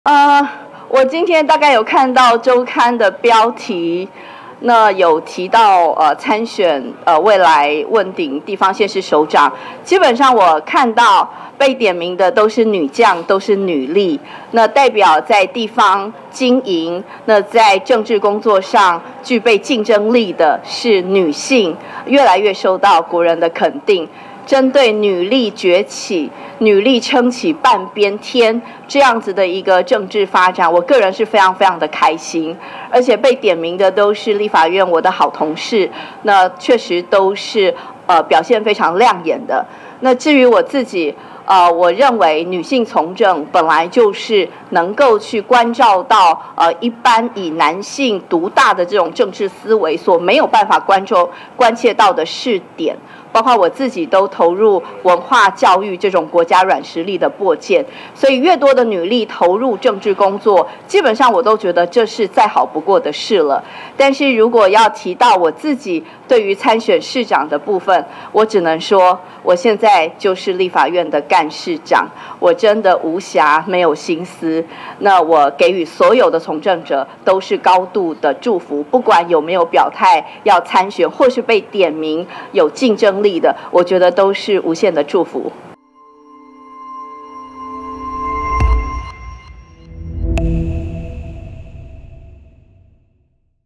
CTWANT 政治新聞 / 傳遭民進黨點名參選台北市長 吳思瑤表態認「無暇也沒有心思」
週刊報導指稱，民進黨有意將縣市長初選時程提前至少半年，並點名多位女將參選。對此，被點名參選台北市長的民進黨立法院黨團幹事長吳思瑤29日受訪表示，被點名的人都是她的好同事，認為有越來越多女力投入政治工作是再好不過的事情，至於她自己現在就是立院黨團的幹事長，強調「真的無暇、也沒有心思參選」，雖然未收到要提前半年舉行初選的訊息，但相信民進黨會推出最適任、最亮眼的人選。